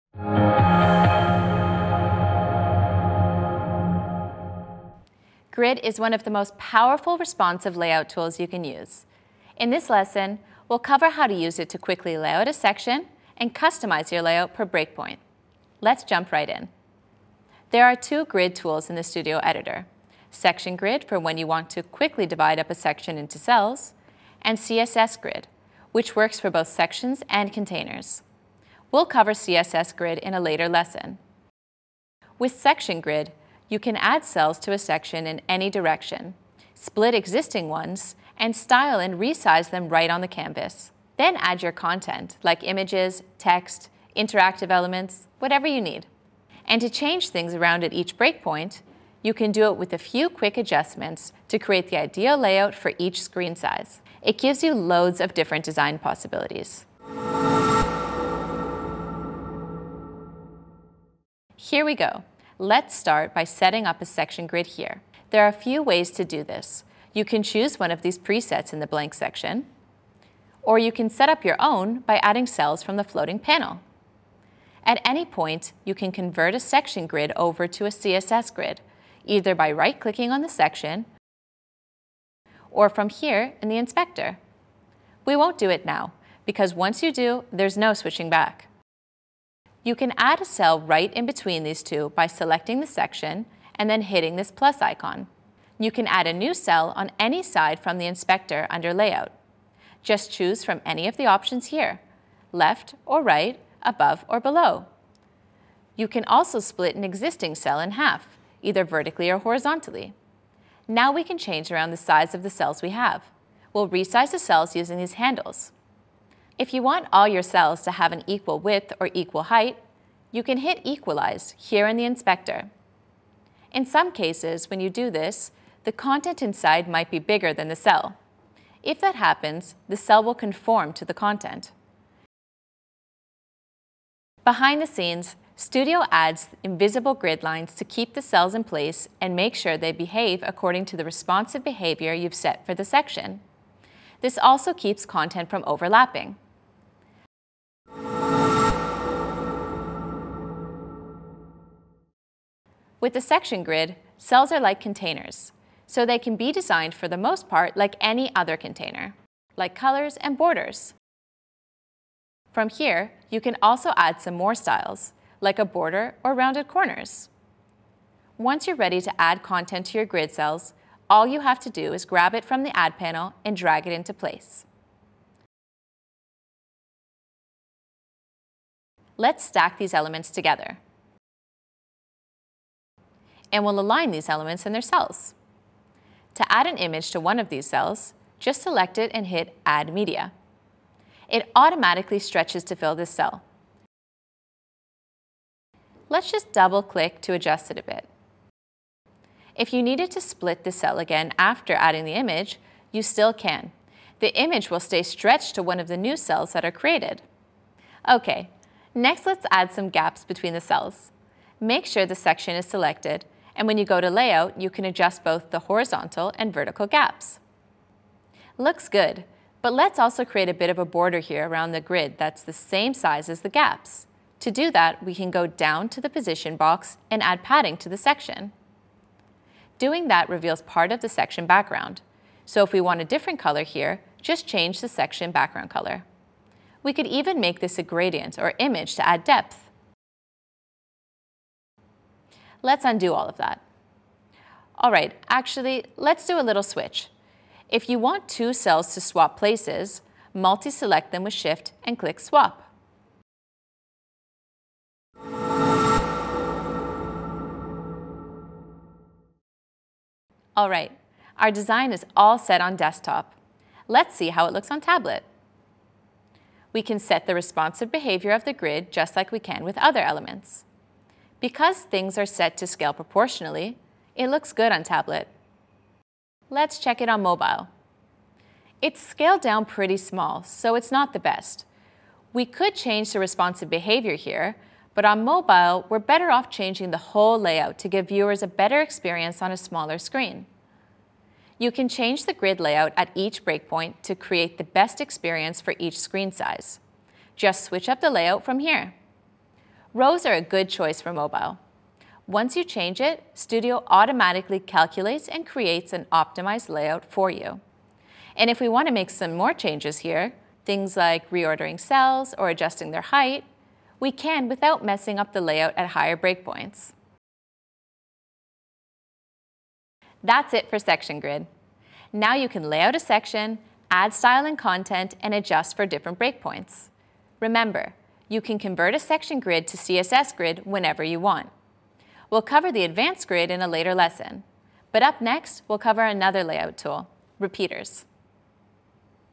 Design with Section Grid with Wix Studio
Learn to design your sections using a Grid.

In this lesson, you’ll learn how to create intricate and unique designs quickly by dividing your sections into cells and positioning your elements according to those cells to keep your design looking its best on each Breakpoint.

0:00 - Intro
0:17 - Grid demonstration
0:57 - Add & set your basic Grid layout
2:28 - Customize Grid cells